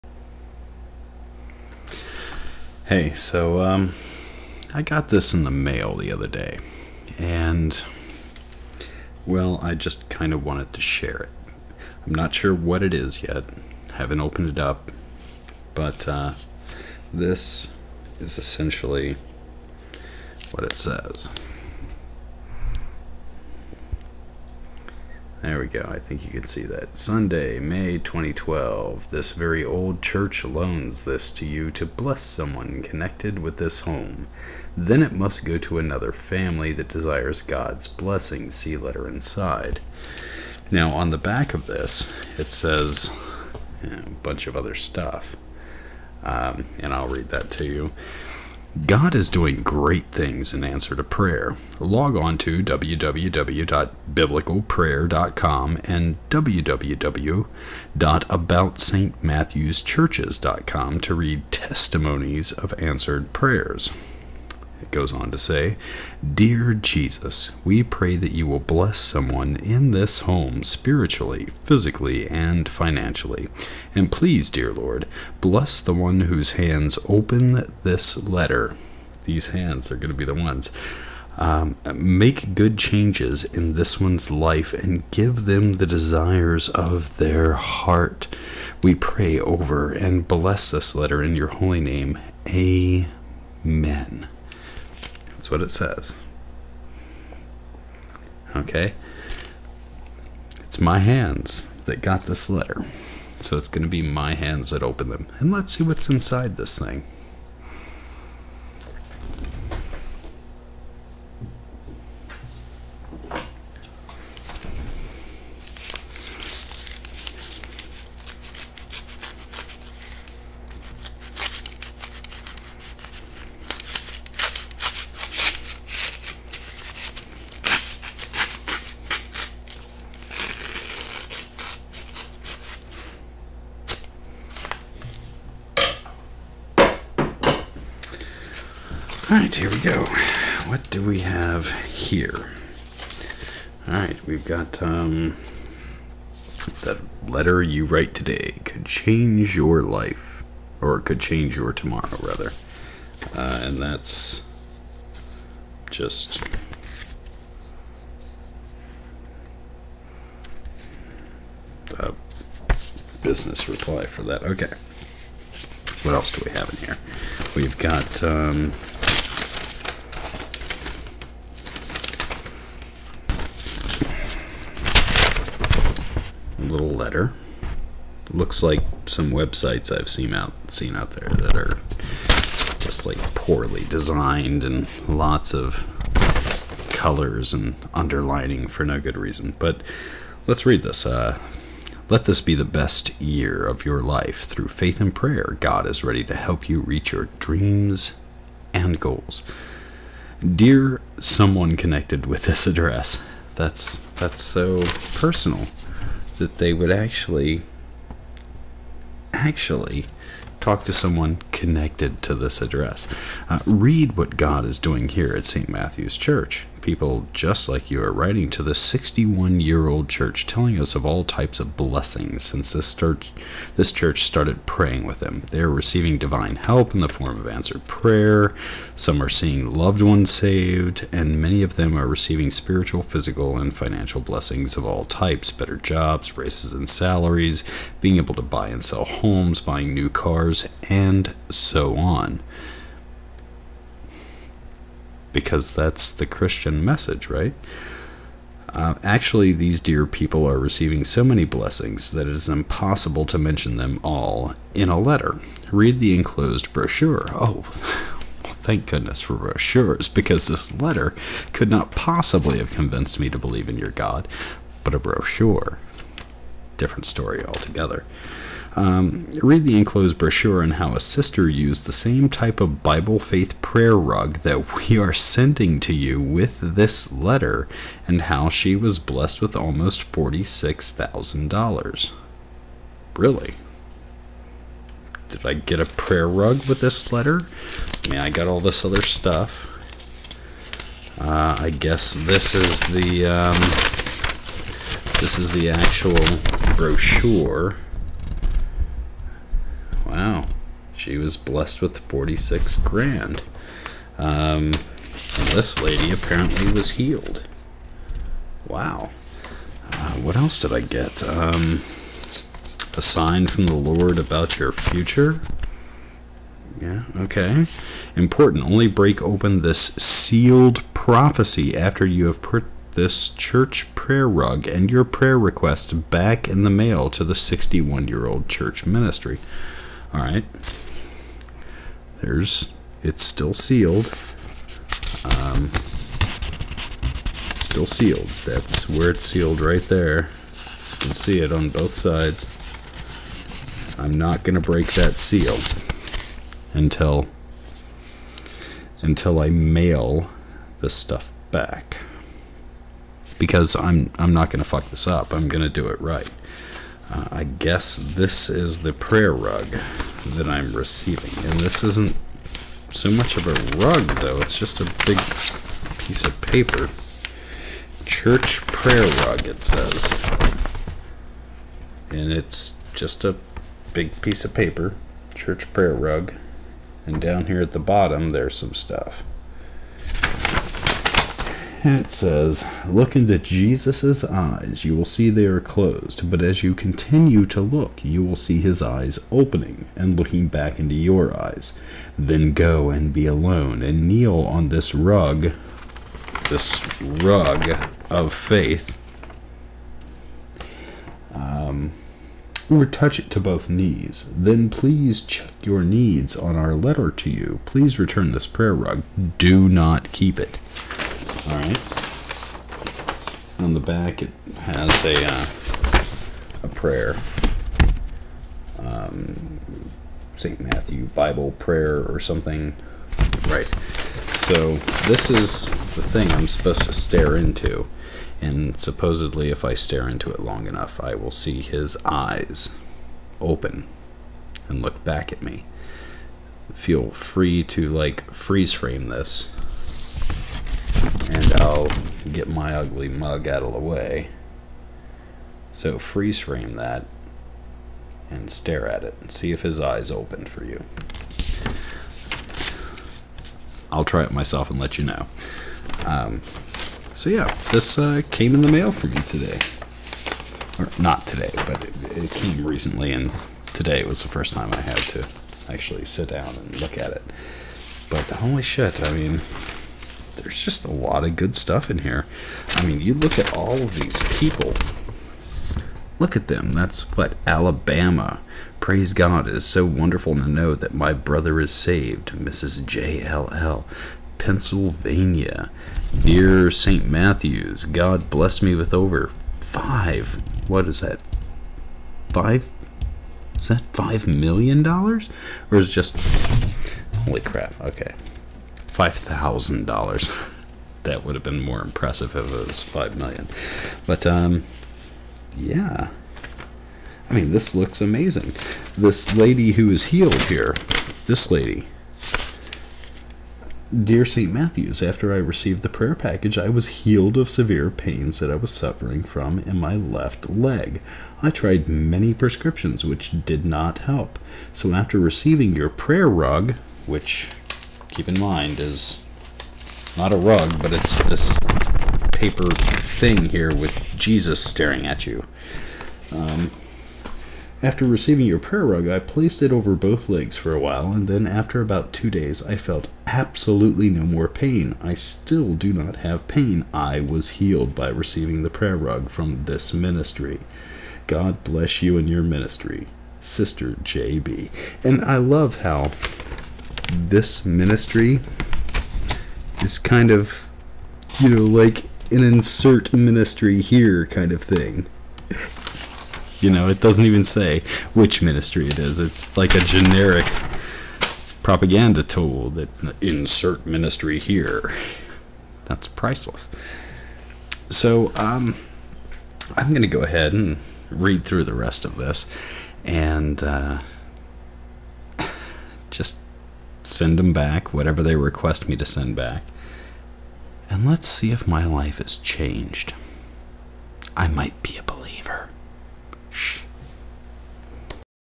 Fun mail from christians!! Prayer Rugs and Cash Prizes!!!
Prayer flyers (I mean "rugs") business reply envelopes, (wait...a business??!!) messages of lottery winings and cars and homes!!! Very little about salvation though. bummer.